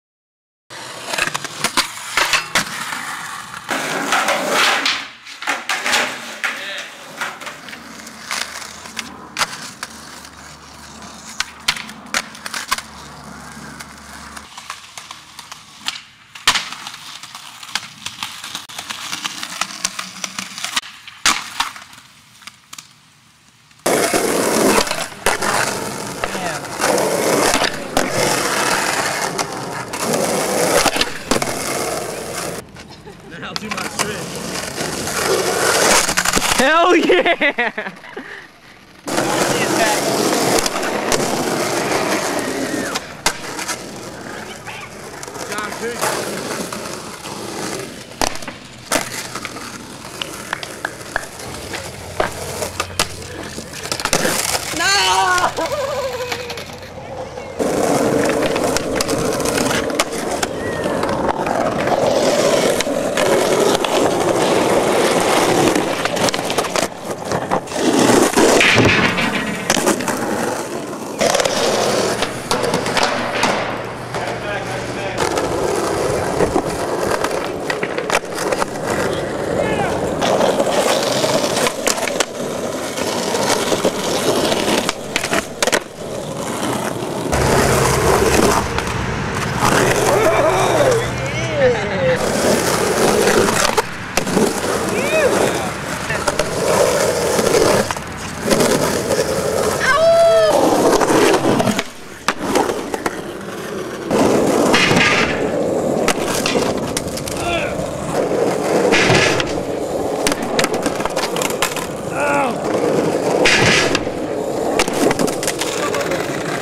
Extra Tricks for Framework (7/6/10)
Featuring Zach Panebianco, Pat Hennigan, Joey Christoffers, Pat McCullion, Anthony Krystopa, Alex Robinson, Dustin Detweiler, Kevin Pham, Billy Simpson, Jimmy Simpson, Collin Bauder, Jeff Colombo and John Provencher.

Check my blog for the background story and photos